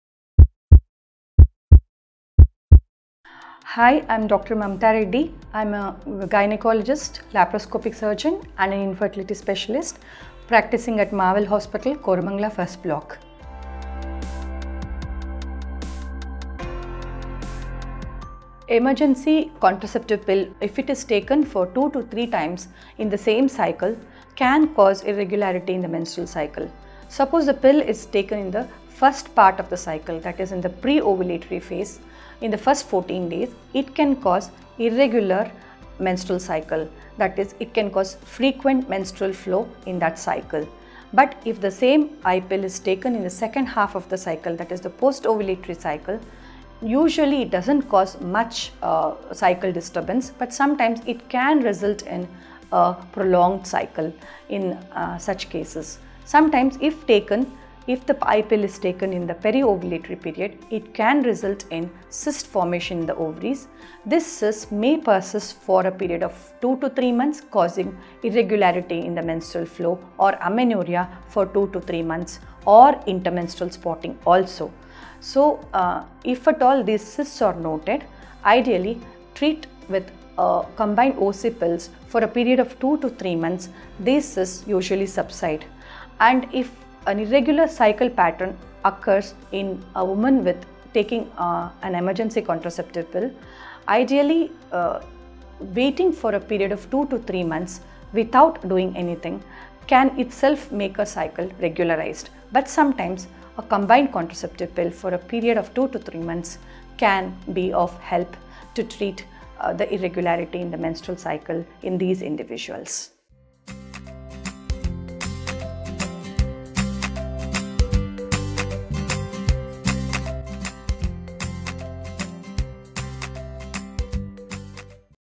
Can consecutive emergency contraceptive pills cause missed periods? - Dr. Mamatha Reddy YV
Emergency contraceptive pill or an i-pill, if it is taken for 2 to 3 times in the same cycle can cause irregularity in the menstrual cycle. Suppose the pill is taken in the first part of the cycle, i.e. the pre ovulatory phase, in the first 14 days it can cause irregular menstrual cycle. That is it can cause frequent menstrual flow in that cycle. But if the same i-pill is taken in the second half of the cycle i.e. the post ovulatory phase usually it doesn’t cause much cycle disturbance, but it can result in prolonged cycle in such cases. Sometimes if the i-pill is taken in the peri ovulatory period it can result in cyst formation in the ovaries. This cyst may persist for a period of 2 to 3 months causing irregularity in the menstural flow or amenorrhea for 2 to 3 months or intra menstrual spotting also. So if at all the cysts are noted ideally treat with combined OC pills for a period of 2 to 3 months. If a irregular cycle pattern occurs with a woman taking an emergency contraceptive pill, ideally waiting for a period of 2 to 3 months without doing anything can itself make a cycle regularised.